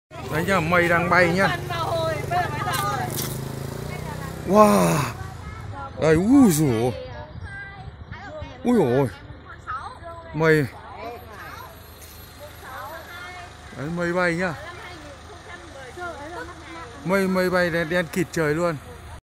Ngắm Mây Trên Đỉnh Núi Ba Vì | Mãn nhãn ngắm Núi Ba Vì bồng bềnh trong mây | Địa Điểm Săn Mây Ba Vì
Mây Ba Vì đẹp nhất là từ tháng 1 đến tháng 12 hàng năm. Bởi khoảng thời gian này, nhiệt độ, thời tiết vô cùng lý tưởng cho sự tích tụ của mây.